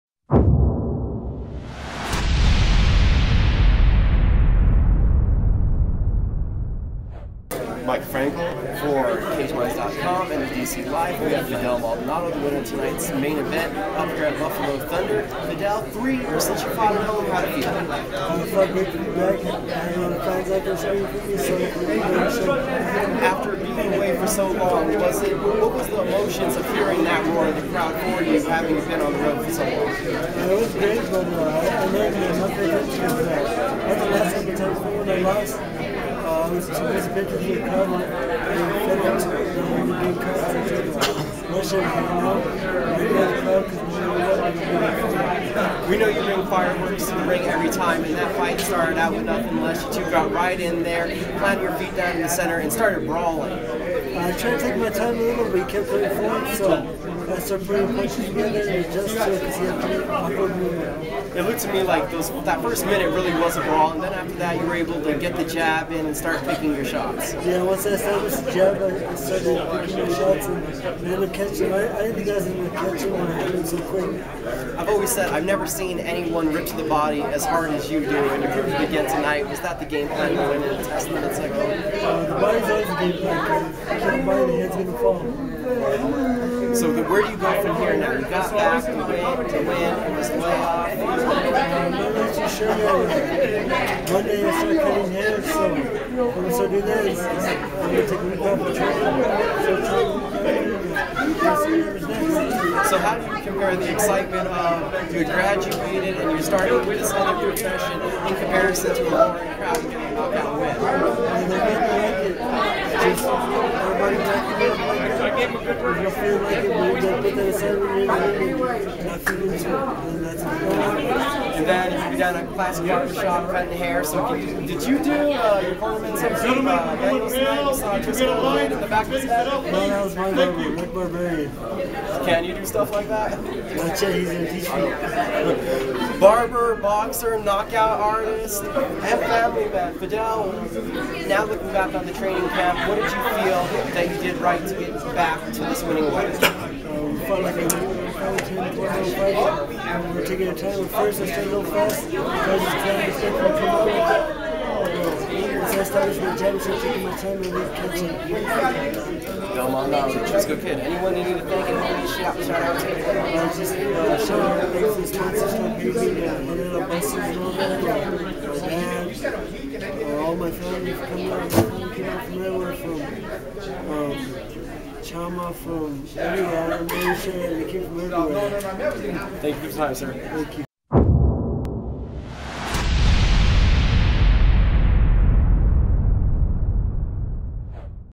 Fidel Maldonado Post "El Futuro" Interview
Fighting back at home in New Mexico for the first time in 3 years, Fidel Maldonado Jr. didn't disappoint scoring a 1st round KO of Lowell Brownfield and we caught up with "The Atrisco Kid" to get his thoughts on getting back in the win column.